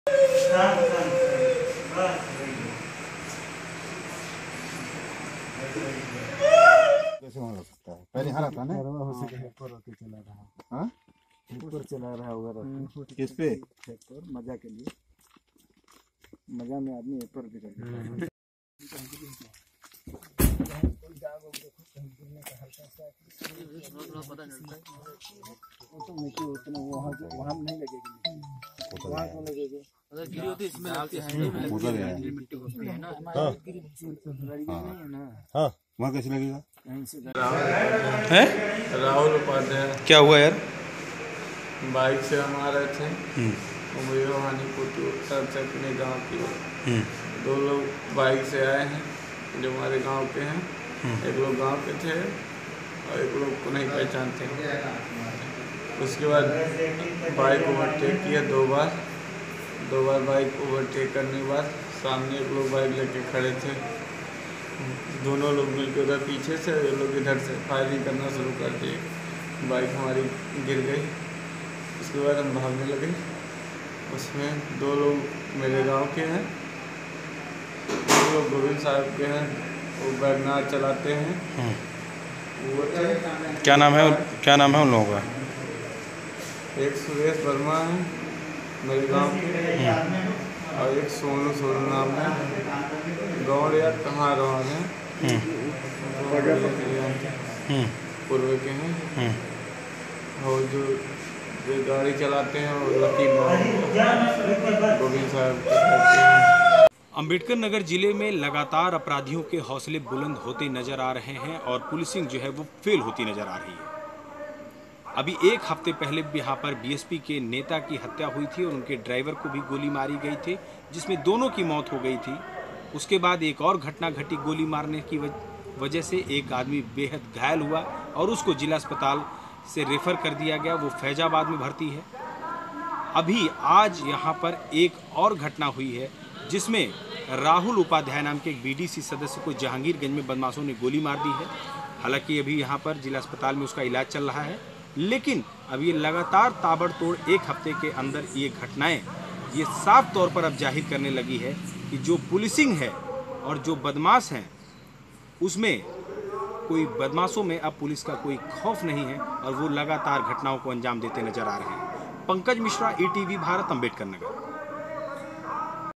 अंबेडकरनगर में बदमाशों का आतंक, एक ही हफ्ते में तीन बार गोलीकांड
अंबेडकरनगर: मंगलवार को ज्वालापुर के जहांगीरगंज में बीडीसी राहुल उपाध्याय पर बदमाश ताबड़तोड़ फायरिंग कर फरार हो गए. गोली राहुल उपाध्याय के पैर में लगी, जिसे जिला अस्पताल में भर्ती कराया गया है. मौके पर पुलिस ने छानबीन शुरू कर दी है.